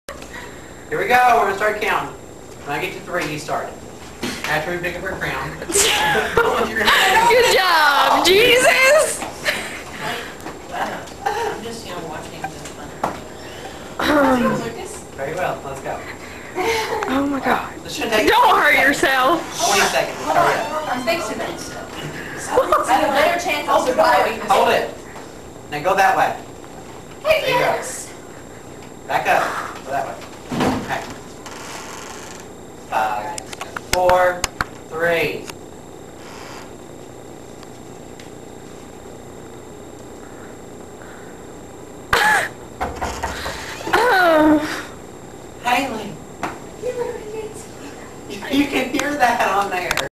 Lucas and Jessie
Lucas and Jessie trying to film for this project and me aparently messing it up...
:P